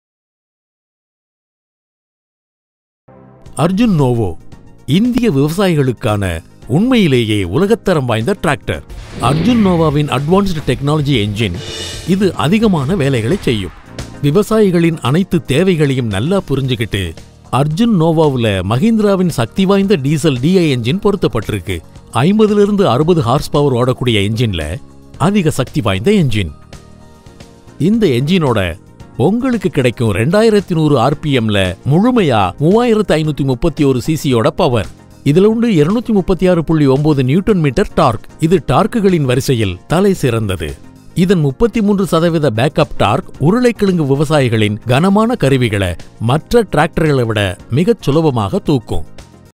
The Hi-Tech Di Diesel Engine - Mahindra Arjun Novo (Walk Around - Tamil)
With Hi-tech powerful Di diesel engine, Arjun Novo is one of the best tractors among the 50-60 HP category tractors. Watch the video to learn more about Arjun Novo’s engine now!

Arjun Novo was launched in the year 2014 by India’s largest tractor maker company, Mahindra & Mahindra. It became the first tractor to be rolled out of the higher HP platform, has been developed through extensive research and insights conducted with over 100 farmers across 10 states to understand the mechanization needs of farmers. The tractors included under Arjun Novo varies from 49.9 HP to 57 HP.

Learn more about the features and benefits of the Arjun Novo

Follow us-